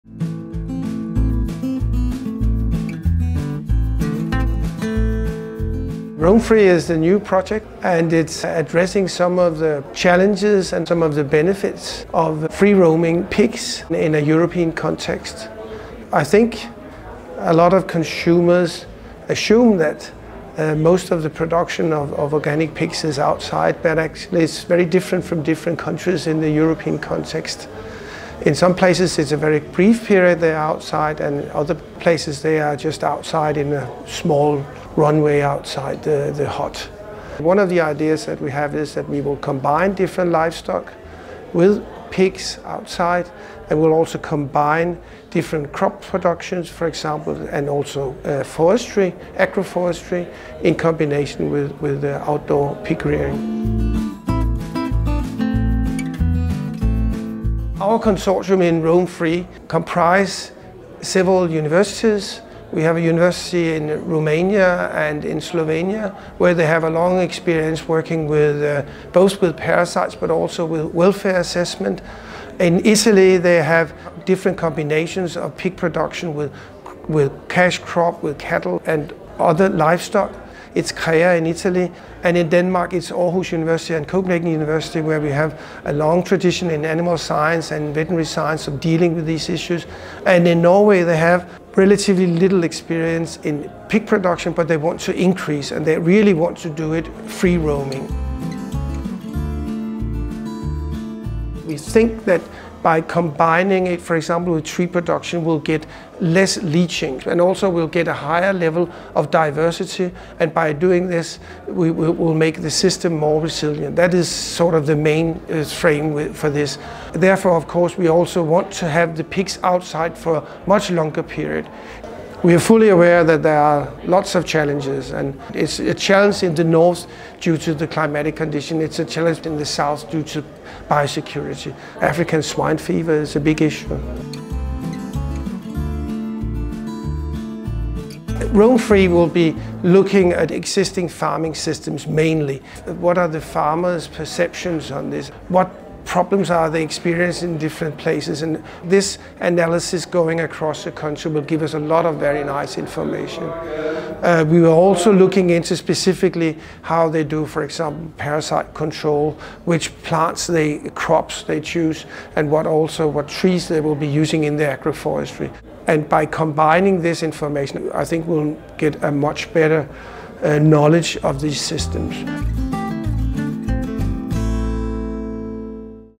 Pig production in sustainable mixed free-range systems (ROAM-FREE CORE Organic)
ROAM-FREE is addressing challenges and benefits of free roaming pigs in a European context. The project combines different livestock with pigs outside. ROAM-FREE will also combine different crops, for example chicory roots, and also forestry, agroforestry in combination with outdoor pig-rearing. Roam-Free will be looking at existing farming systems mainly.